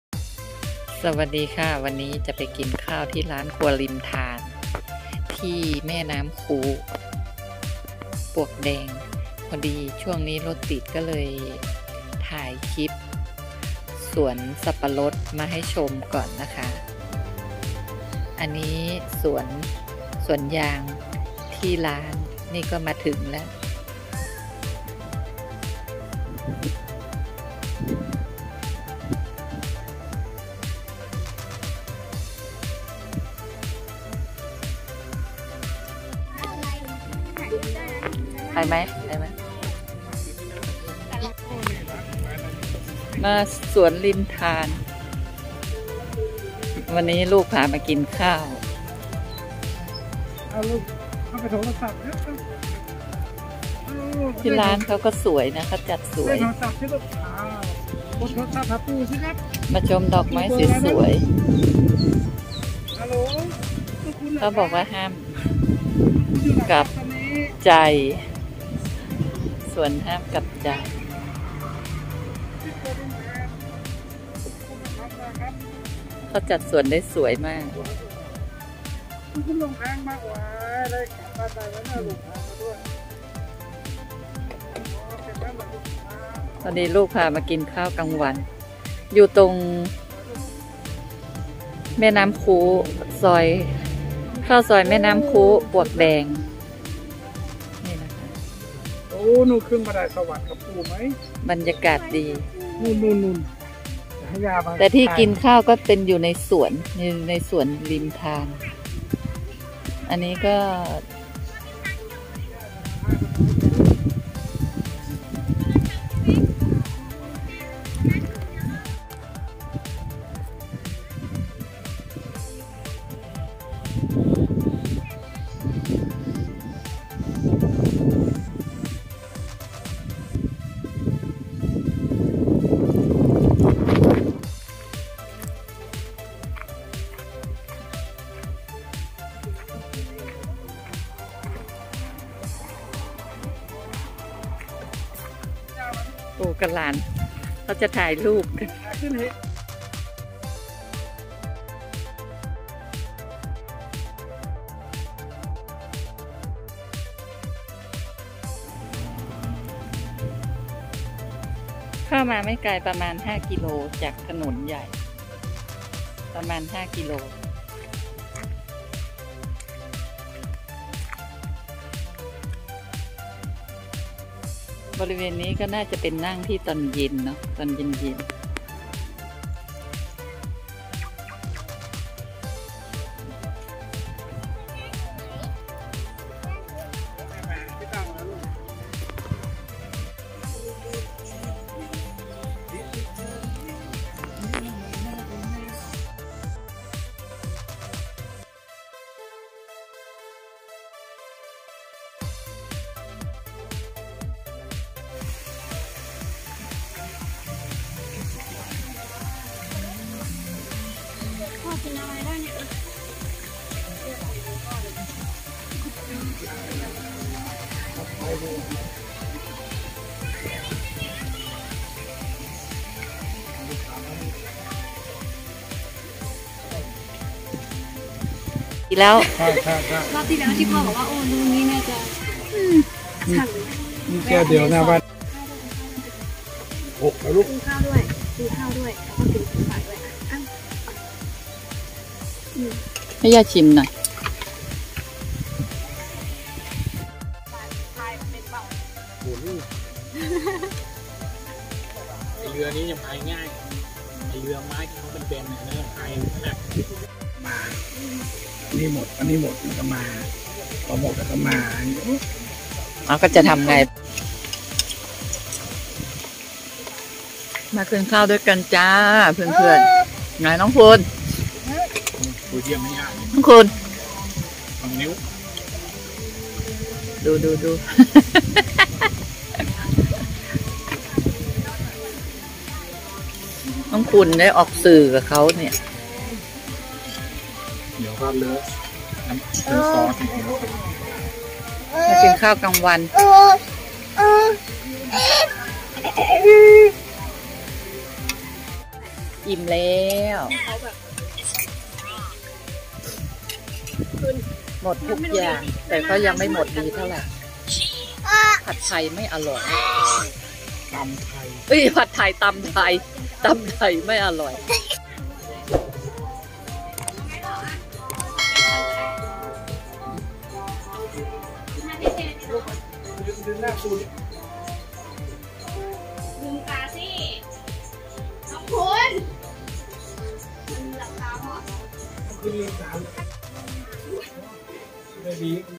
เปลี่ยนมากินข้าวนอกบ้าน และชมบรรยากาศ ดอกไม้สวยๆ
ชมดอกไม้ และบรรยากาศ กินข้าวนอกบ้าน